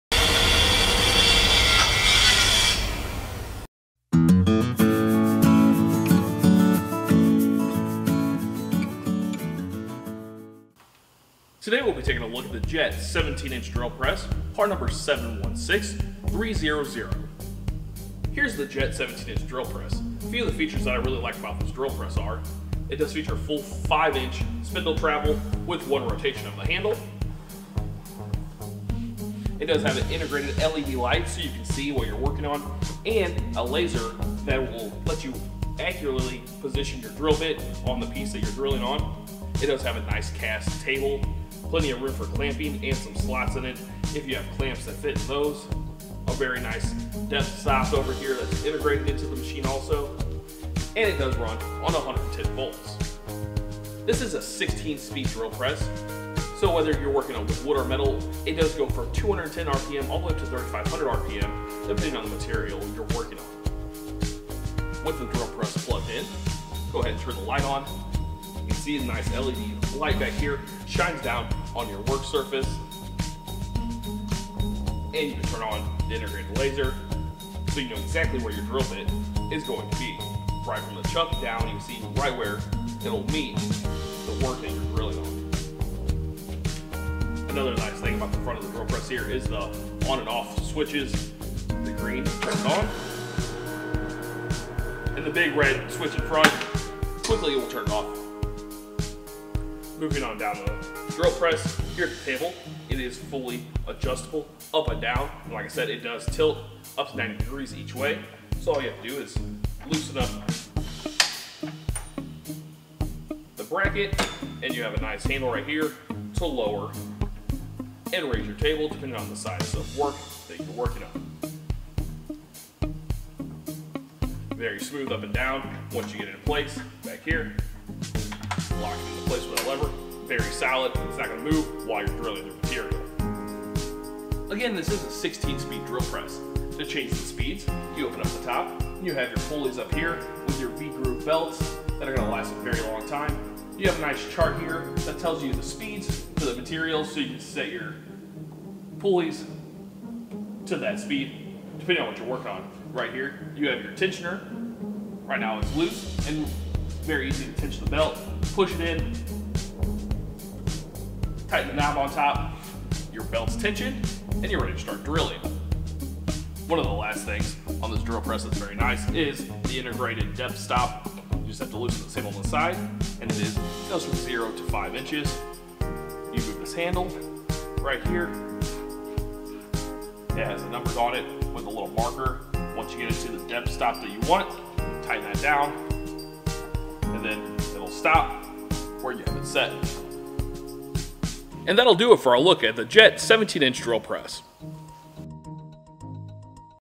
716300 Jet JDP-17, 17" Drill Press
JET JDP-17, a 17" Drill Press, 716300, was designed especially for woodworkers. Its head-mounted dual AC-powered laser guide system provides an accurate drilling cross-point on the workpiece.